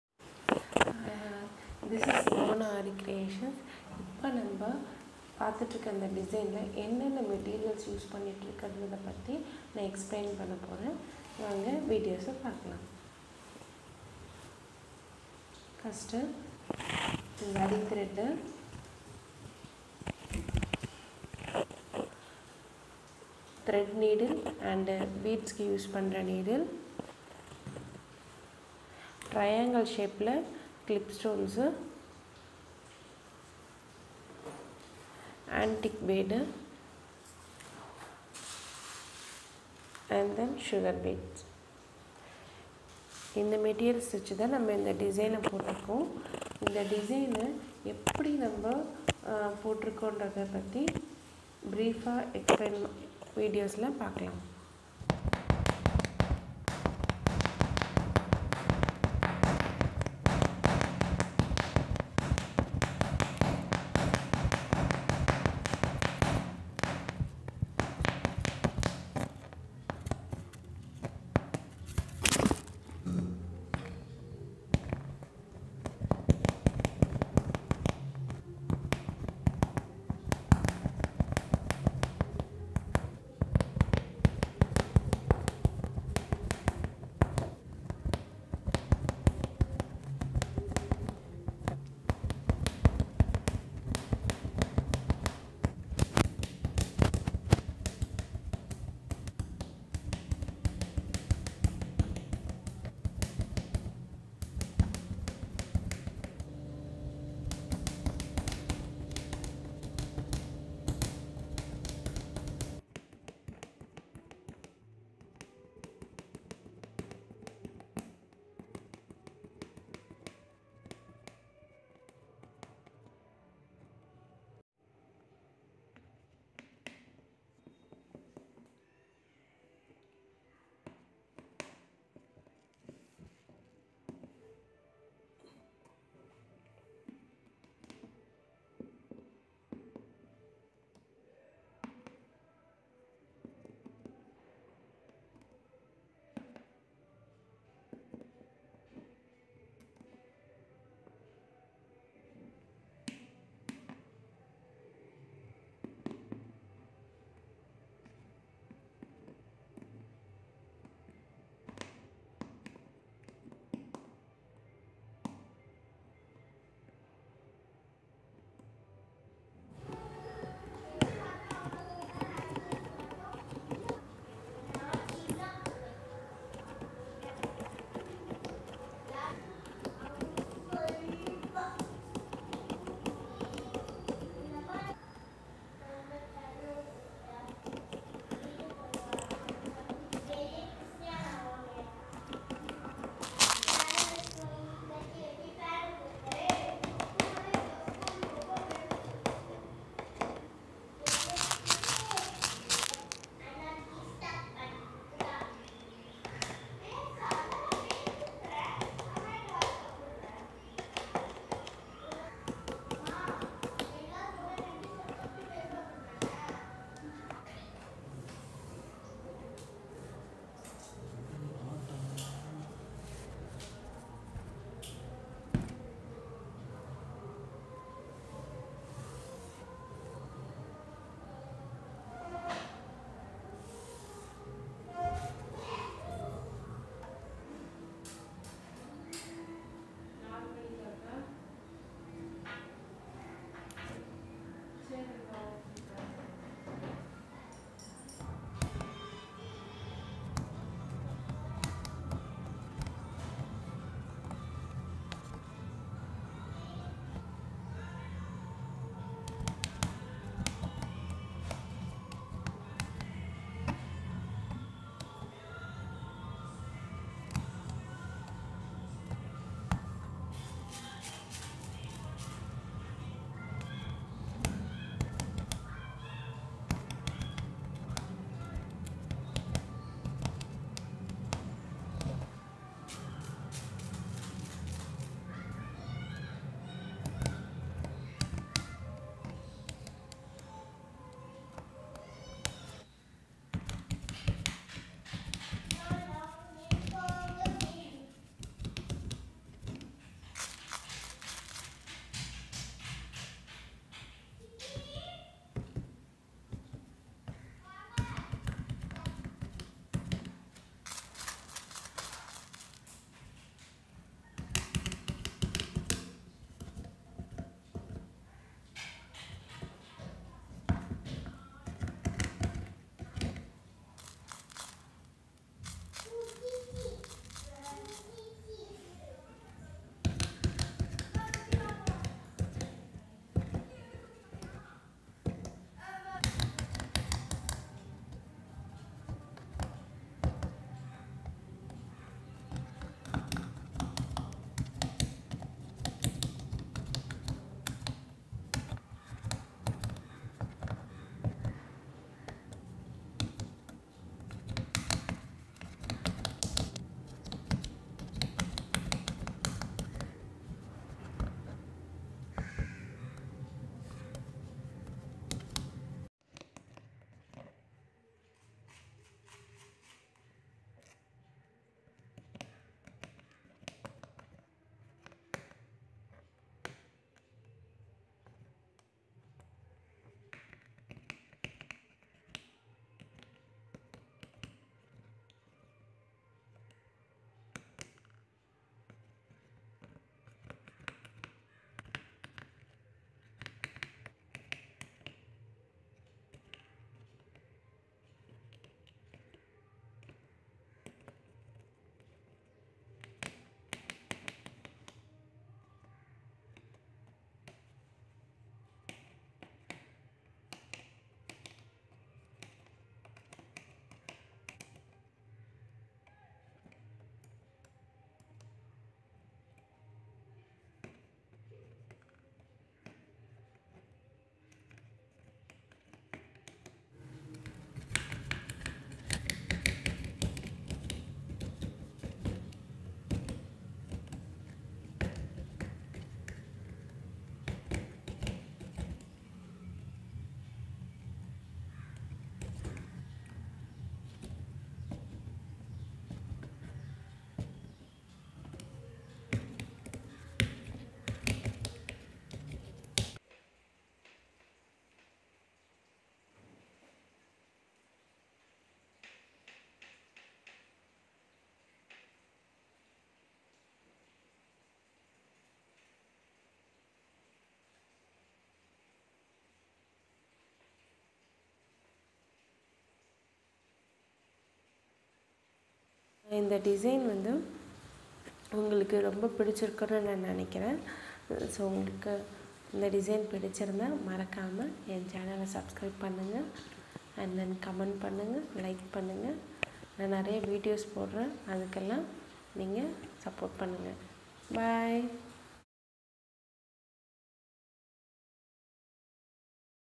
Engagement aariwork blouse/kundan stone blouse design
meterials
zarithread,sugar bead,antic bead no 5,clip stone triangle size 4